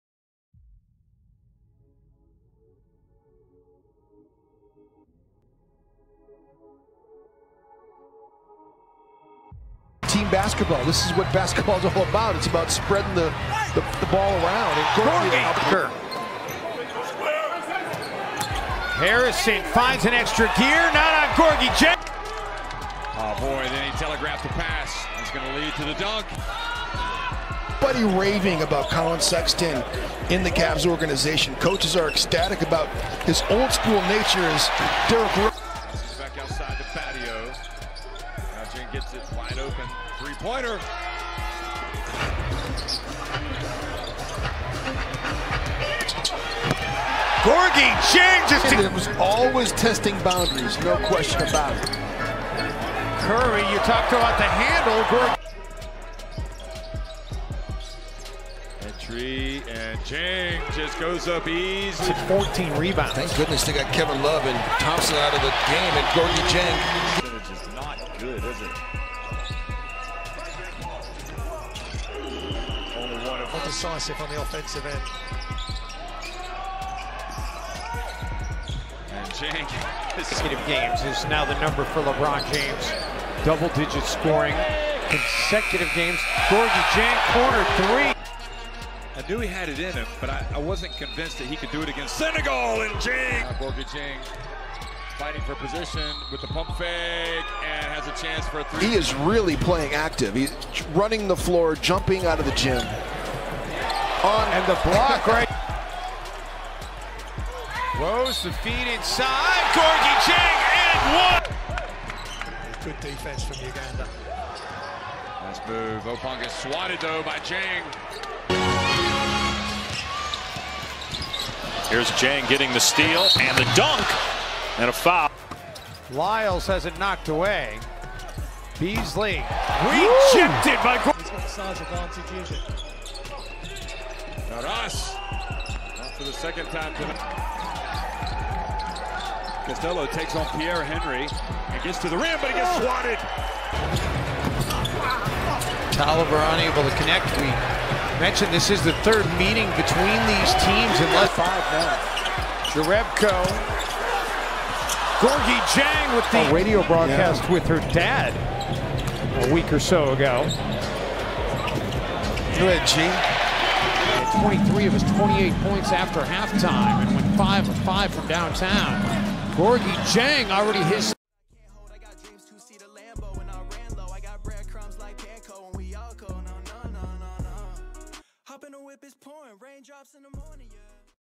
SAN ANTONIO SPURS: Gorgui Dieng ᴴᴰ
BREAKING NEWS || SAN ANTONIO SPURS || GORGUI DIENG HIGHLIGHTS
597 CAREER GAMES
7.5 POINTS PER GAME
5.7 REBOUNDS PER GAME
1.3 ASSISTS PER GAME